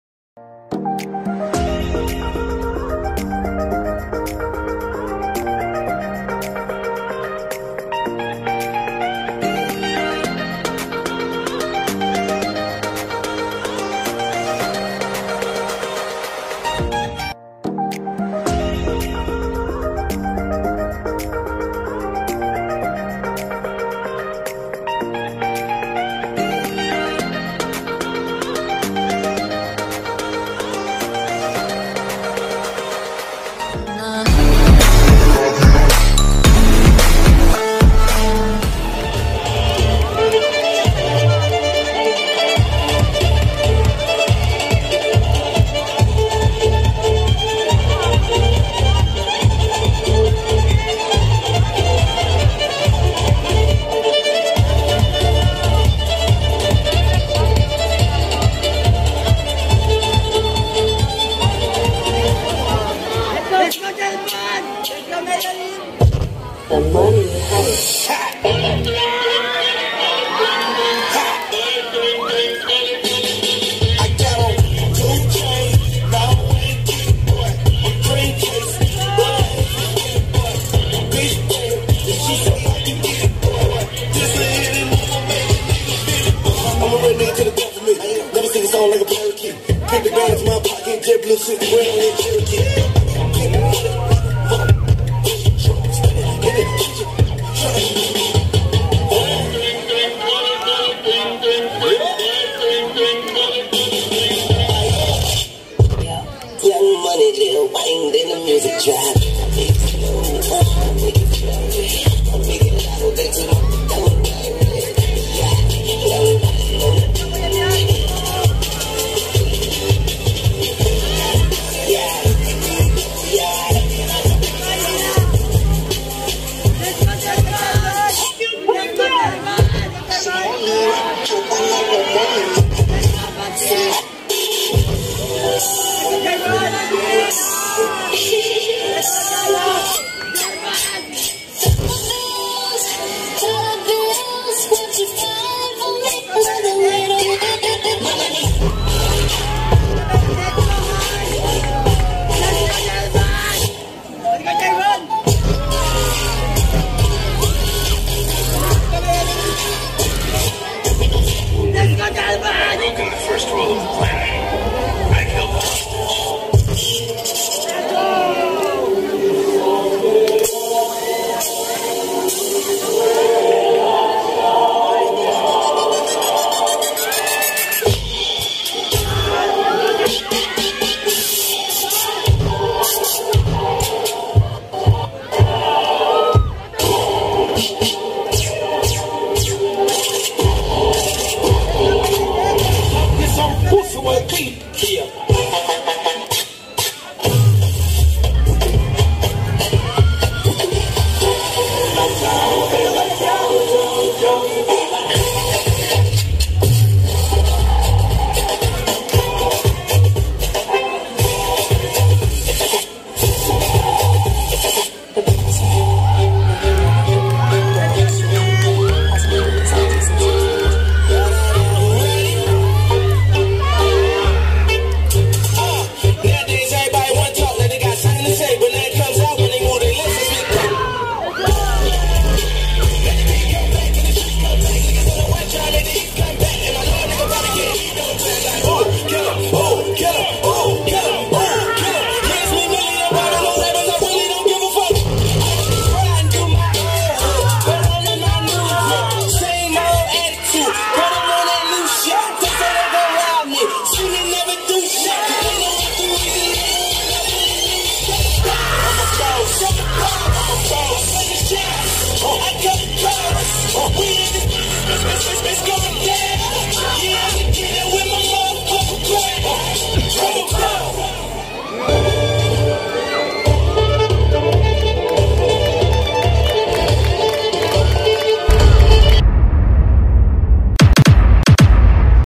XSIS FAMILY DANCE CONTEST BRGY SAN MARTIN DE PORRES Q.C NOV 6,2022
CHAMPION EL REBERDE NEW GEN
1ST PLACE ZIGNO NG ZODIACO
2ND PLACE KENYO HOODZ
CONGRATS TO ALL
DANCE CONTEST